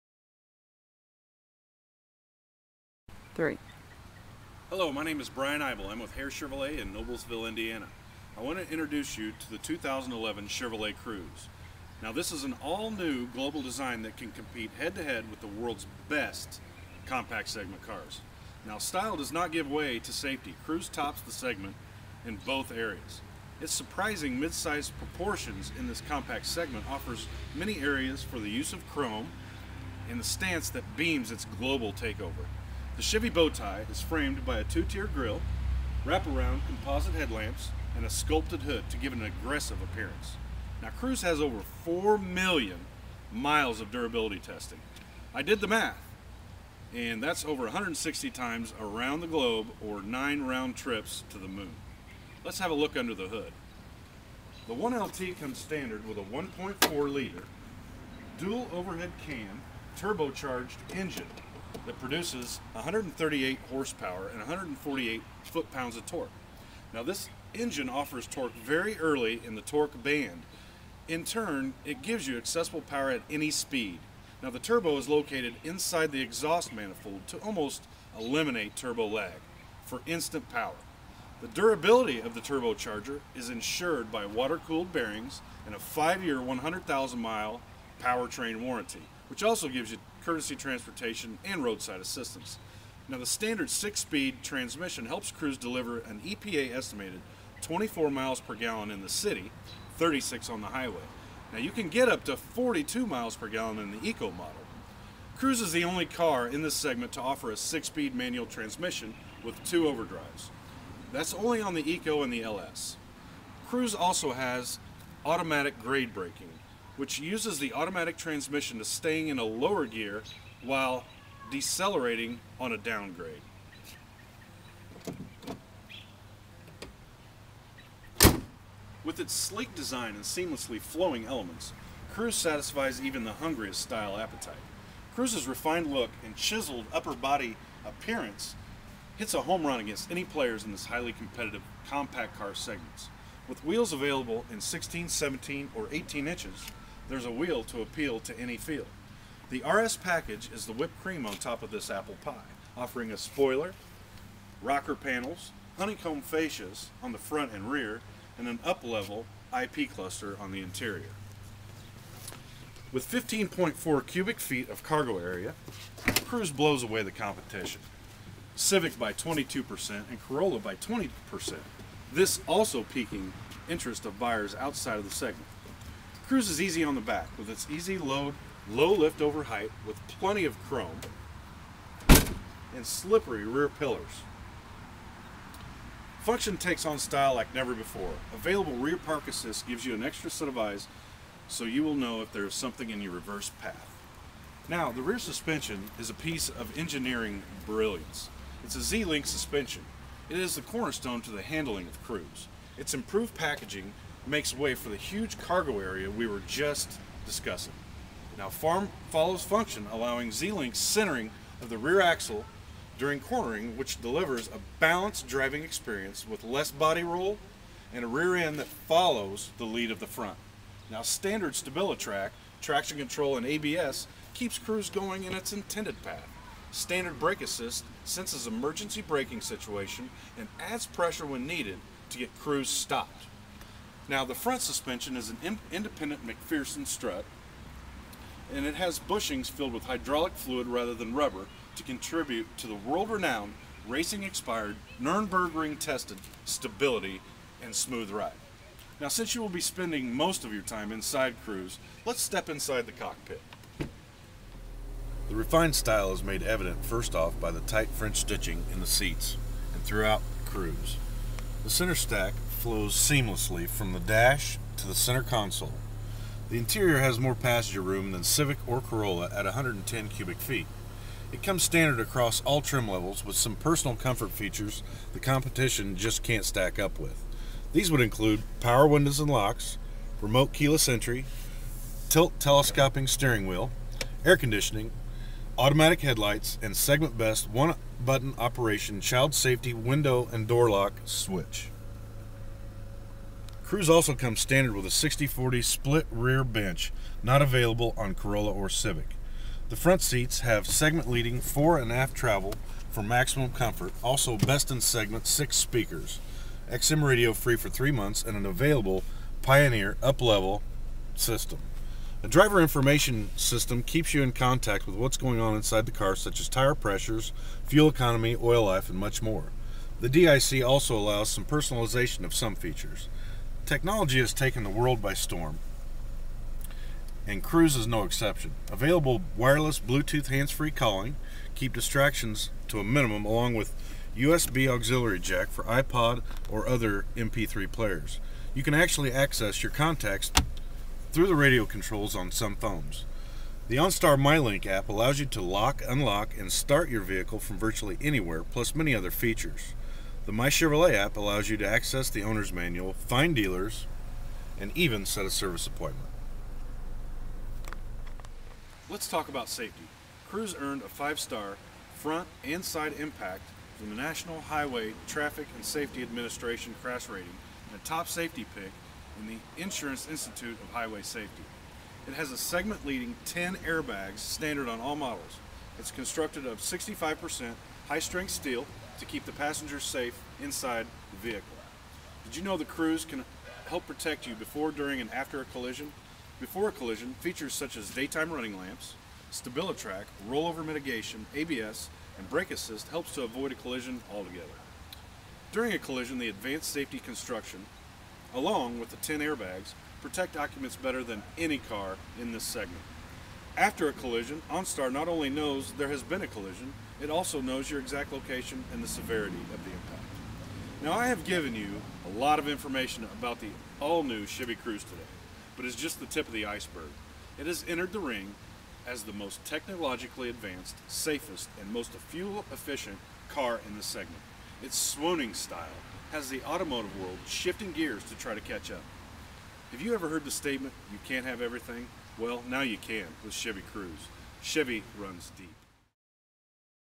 Brian Eybel 2011 Chevy Cruze Walkaround.mpeg
Here is my take on the 2011 Chevy Cruze. Chevy runs deep!